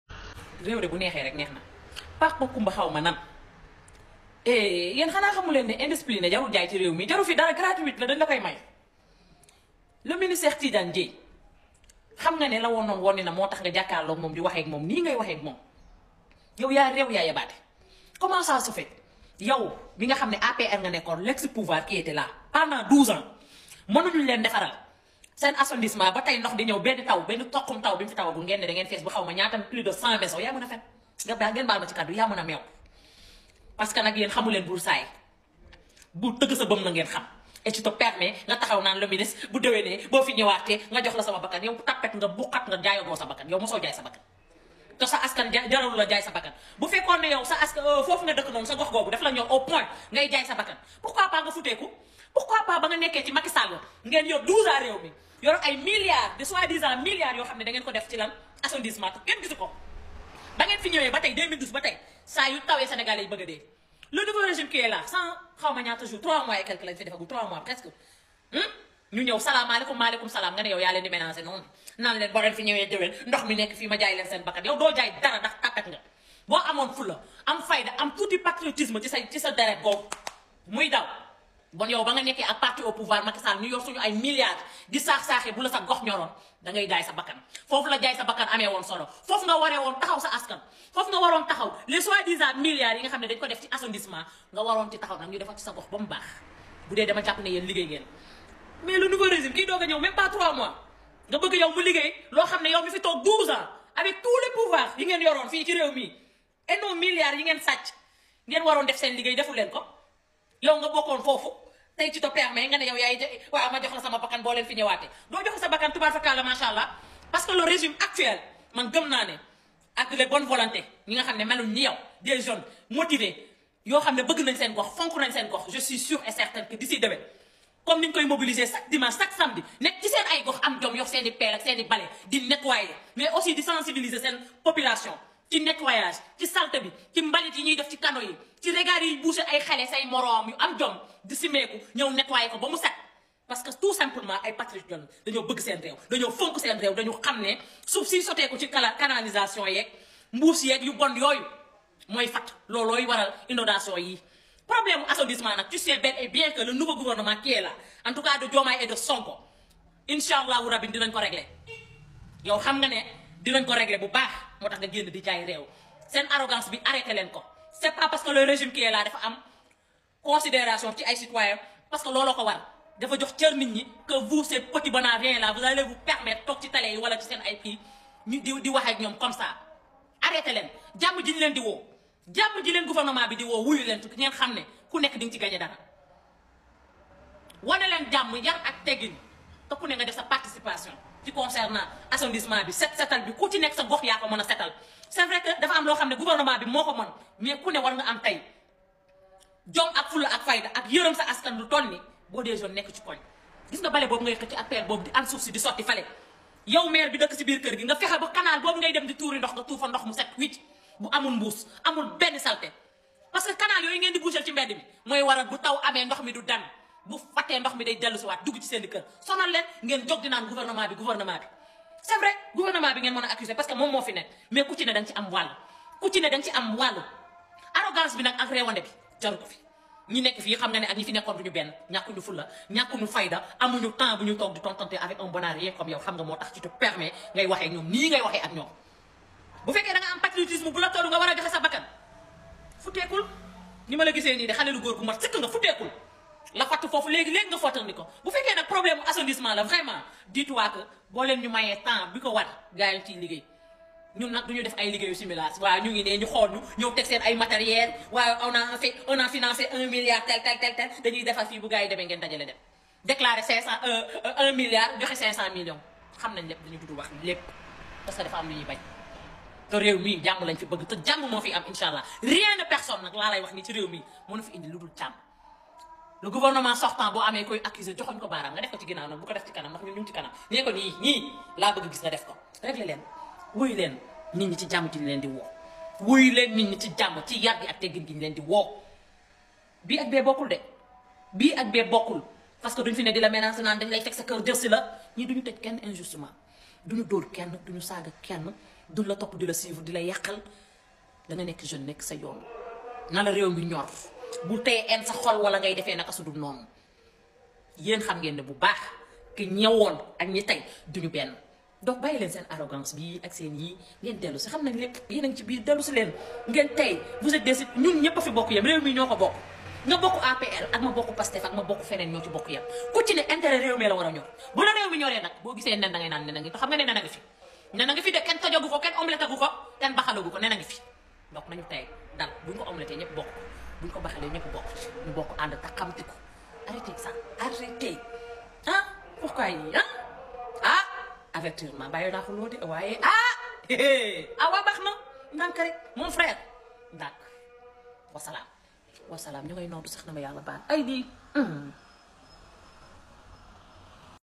Pape Macumba de l'Apr ya rew ya yabaté tu parle avec arrogance avec le ministre Cheikh Tidiane Diey
Pape Macoumba tu parle avec arrogance avec au  ministre de l'assainissement Dr Cheikh Tidiane Dieye